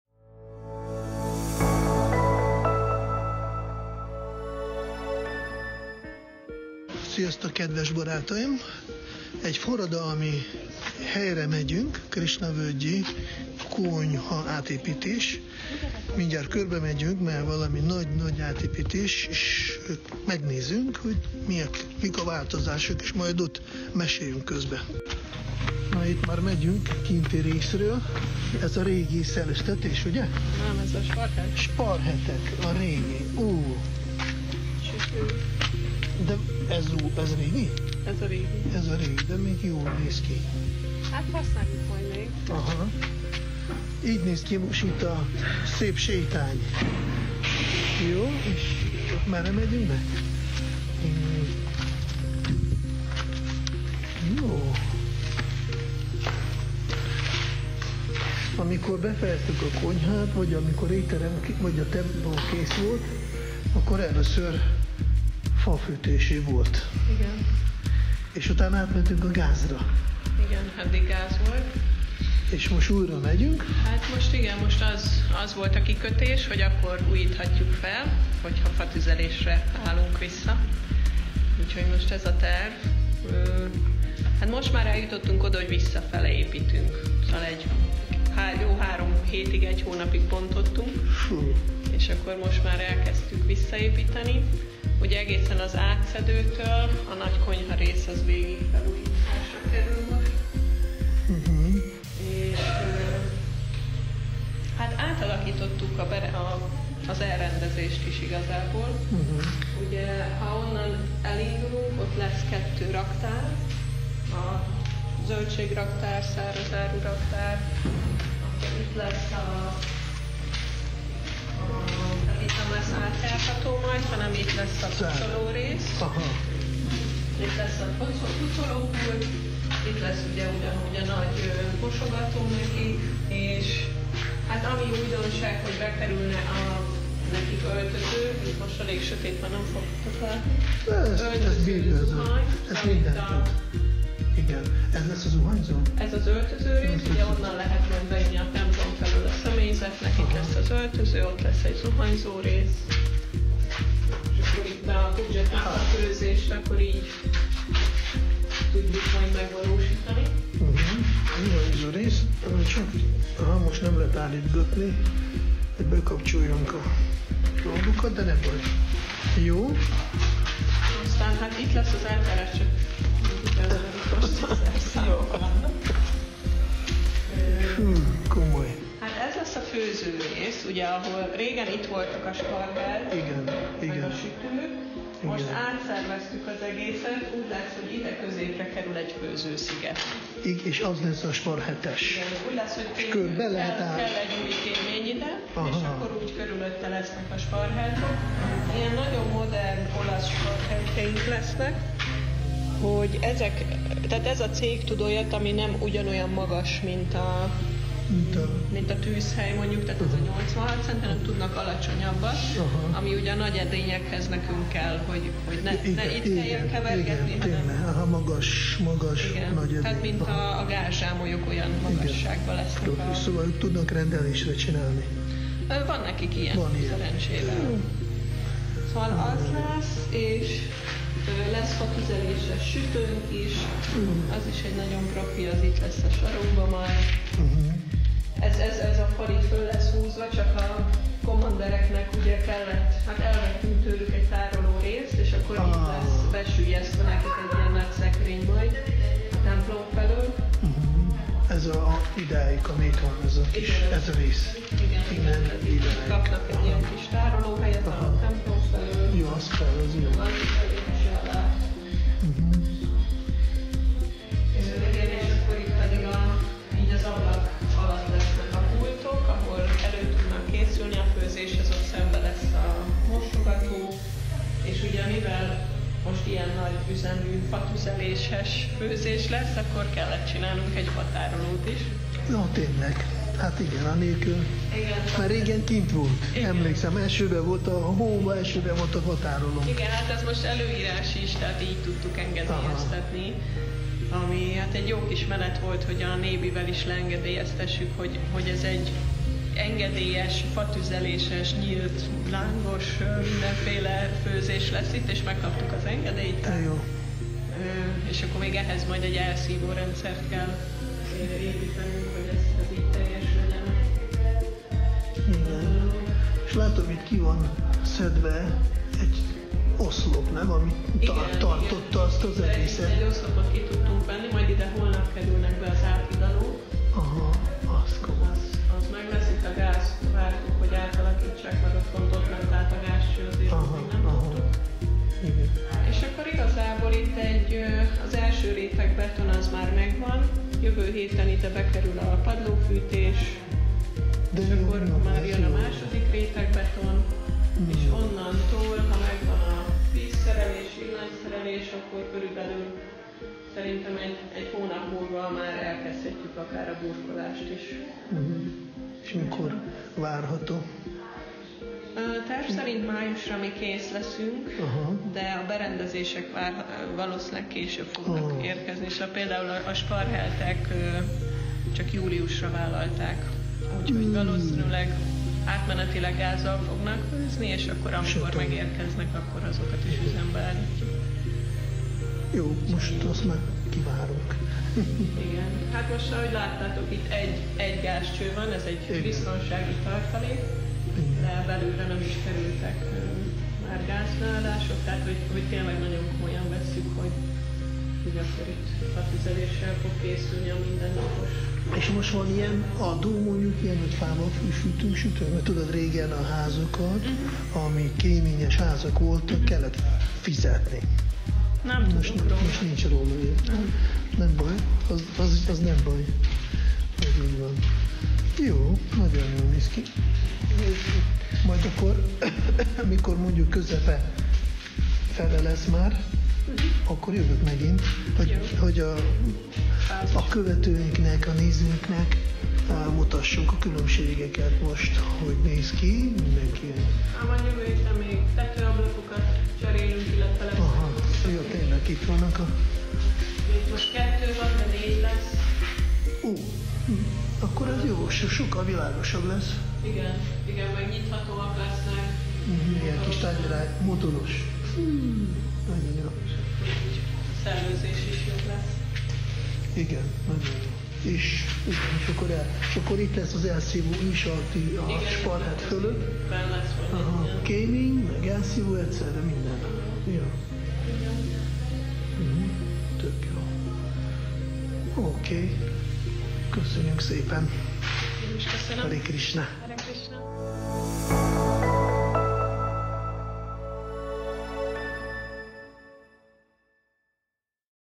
Krisna Völgy - Konyhafelújítás
Bhaktipāda Gosvāmī a Krisna Völgyi konyhafelújítás fejleményei után érdeklődött.
Támogassa a csatornánkat, és exkluzív bónuszokhoz juthat.

Bhaktipāda Gosvāmī